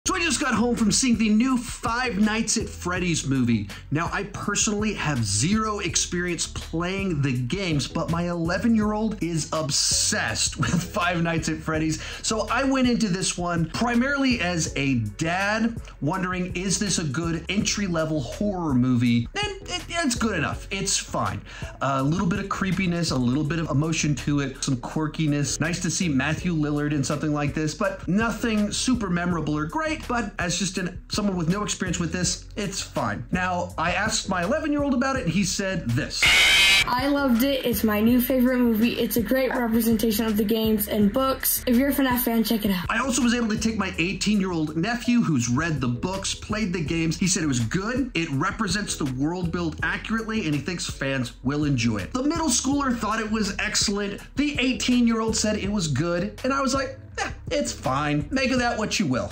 Five Nights at Freddy's Out of the Theater Reaction (FNAF)
I was able to check out Five Nights at Freddy's  FNAF with my son and nephew.  Here's my initial thoughts.

Directed by Emma Tammi
Screenplay by
Scott Cawthon
Seth Cuddeback
Emma Tammi
Story by
Scott Cawthon
Chris Lee Hill
Tyler MacIntyre
Based on Five Nights at Freddy's
by Scott Cawthon
Produced by
Scott Cawthon
Jason Blum
Starring
Josh Hutcherson
Elizabeth Lail
Piper Rubio
Mary Stuart Masterson
Matthew Lillard
Cinematography Lyn Moncrief
Edited by
Andrew Wesman
William Paley
Production
companies
Blumhouse Productions[1]
Scott Cawthon Productions[1]
Striker Entertainment[a]
Distributed by Universal Pictures[1]

VIDEO SUMMARY
This video contains Sean Chandler Talks About's Five Nights at Freddy's Out of the Theater Reaction

Five Nights at Freddy's Reaction
Five Nights at Freddy's Review
Five Nights at Freddy's Movie Review
FNAF Review
FNAF Movie Review